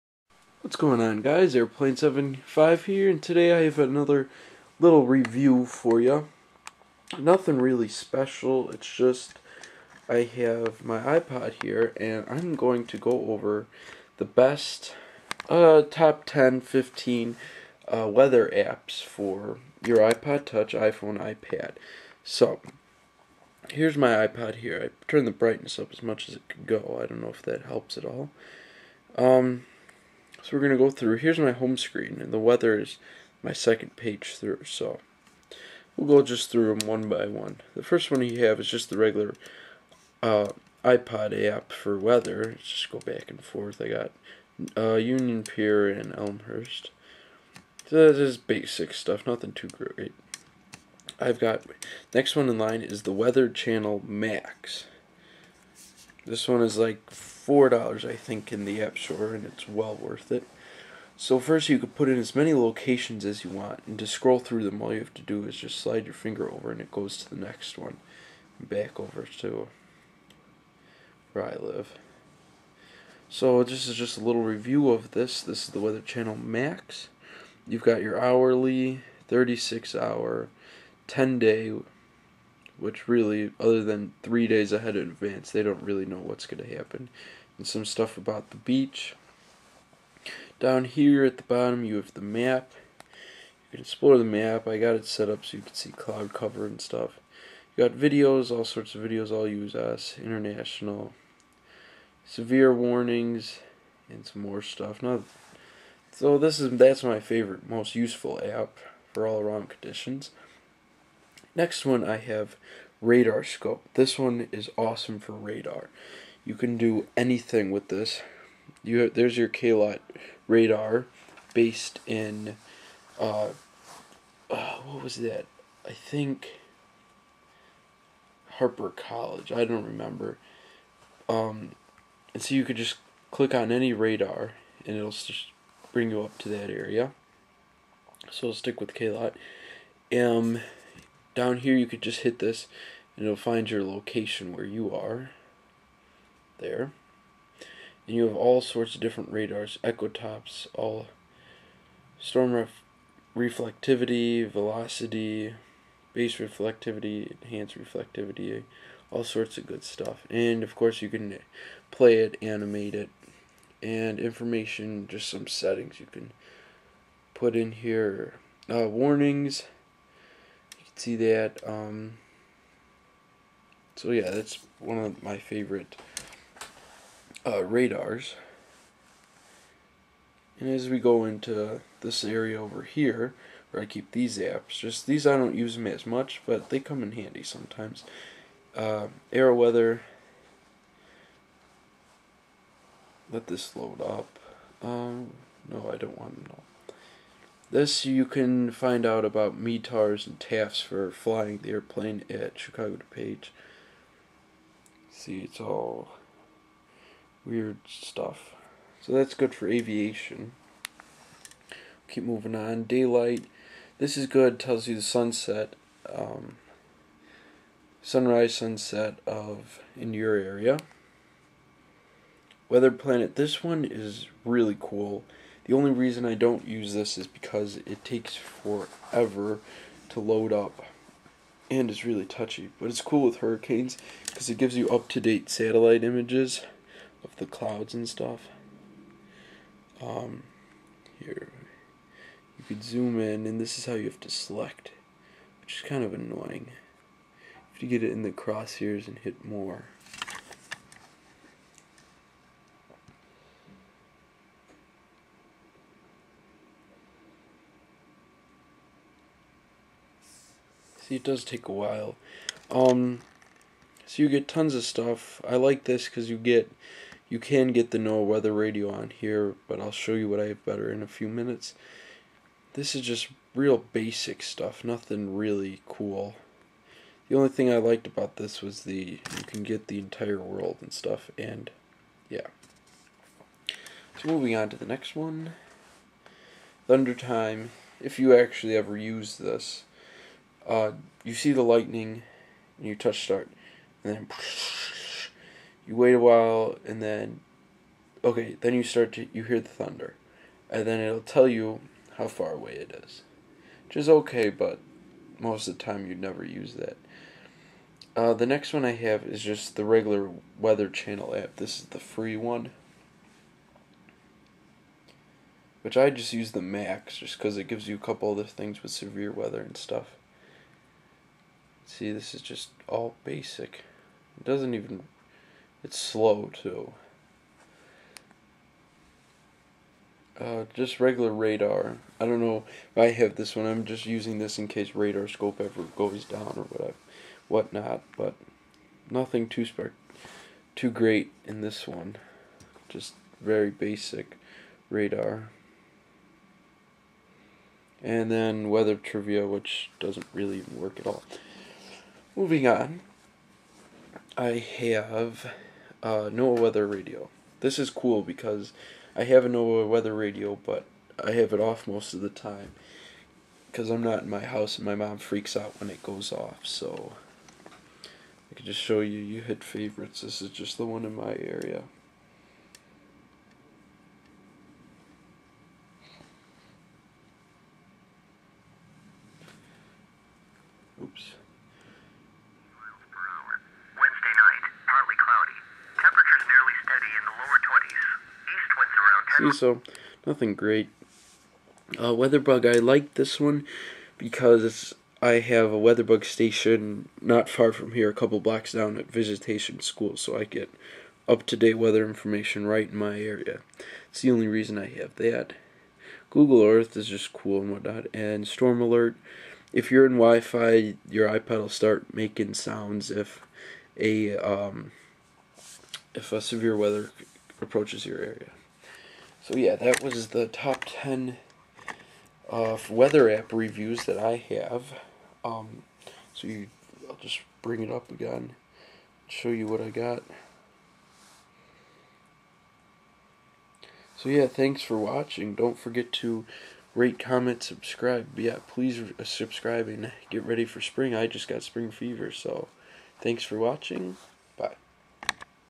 Top Ten Weather Apps for iPod/iPhone/iPad with Review
This is my top ten list of my fav. weather apps i use when storm chasing.
Thanks

Weather, The Weather Channel Max, RadarScope, AeroWeather, Daylight, wxPlant, ThunderTime, TWC, Radar, Radio NOAA, Weather Bug and Storm Alert